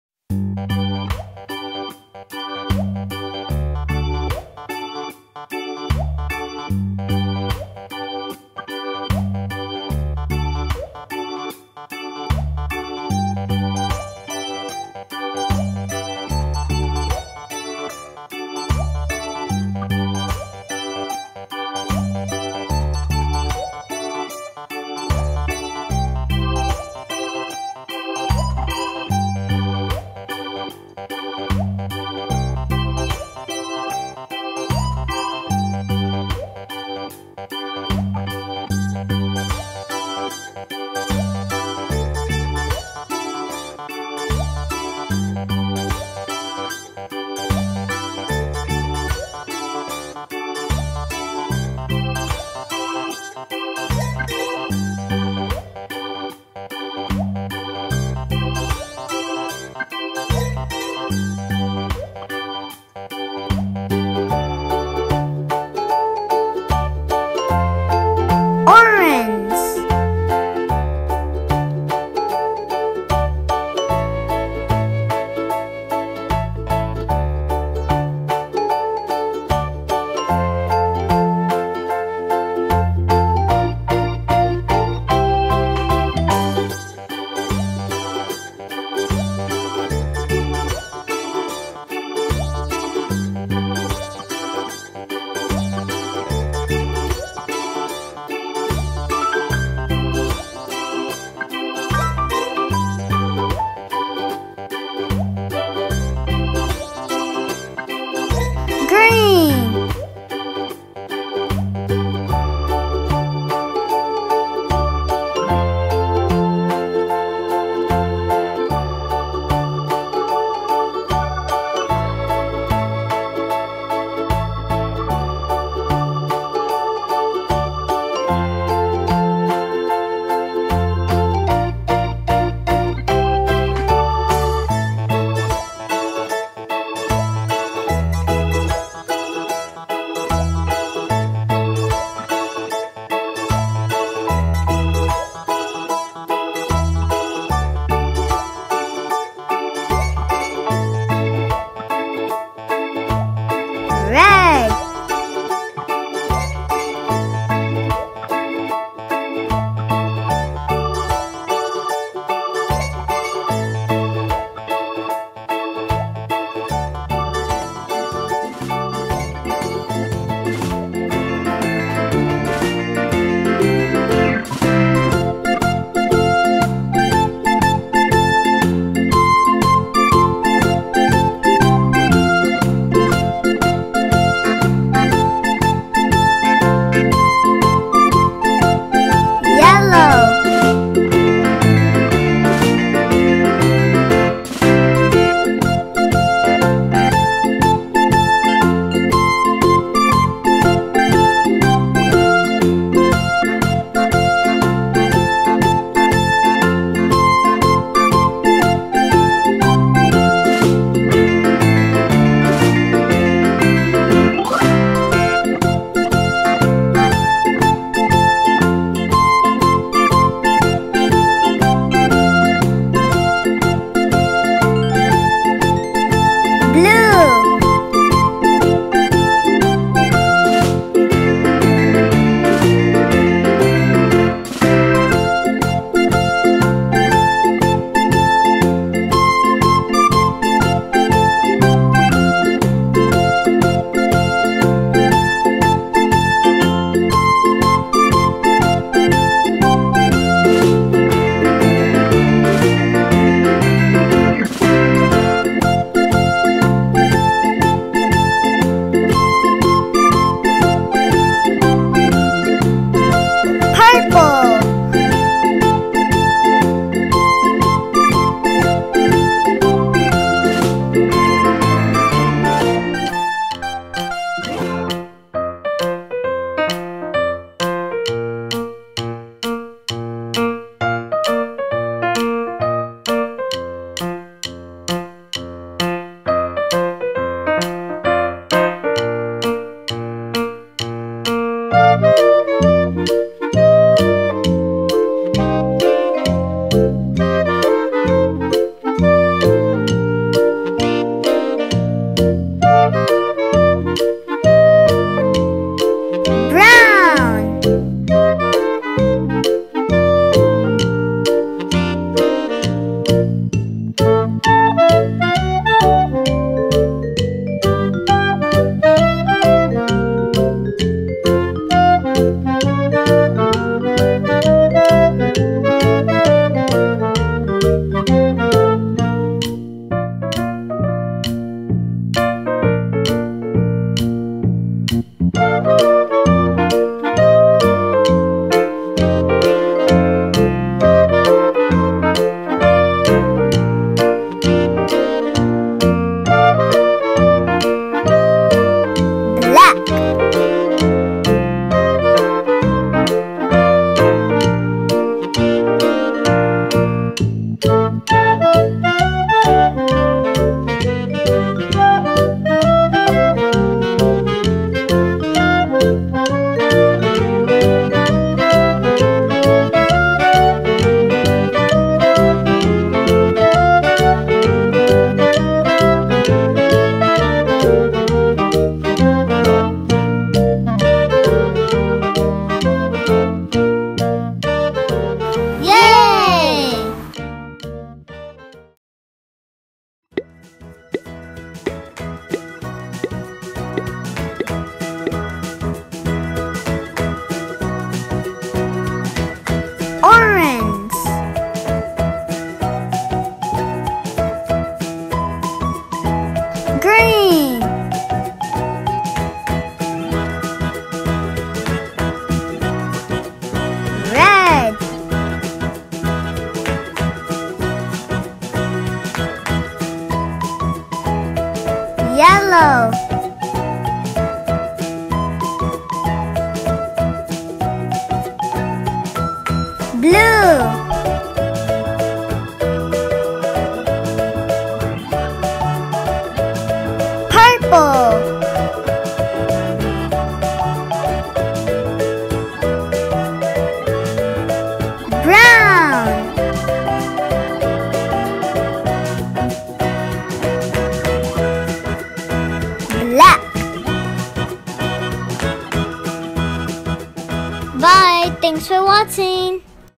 Learn Colors with Play Doh Shape Molds - Fun & Creative for Kids learning words - Nursery Music
Hello!

This is our first video. Our goal with this video is to help parents teach their kids colors while playing with playdoh.

-Learn Colors
-Learn Shapes
-Learn with play-doh
-Learning videos with playdoh
-learn colors wth play doh shapes